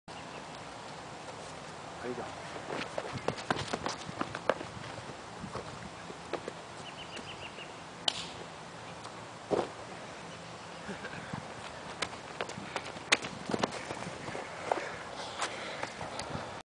synchro flips
on a day of streetboarding you gotta take some time off to have fun